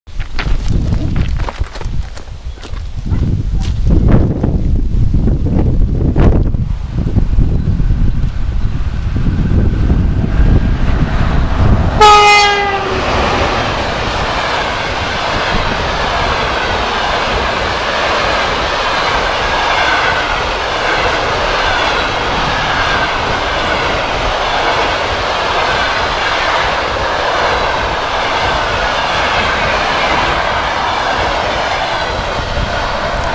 ЭП1М-662 с поездом Новосибирск - Адлер
Электровоз ЭП1М-662 поездом №139 Новосибирск - Адлер, о.п. Большой Лог, перегон Александровка - Кизитеринка, Ростовская область, СКЖД, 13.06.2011 г.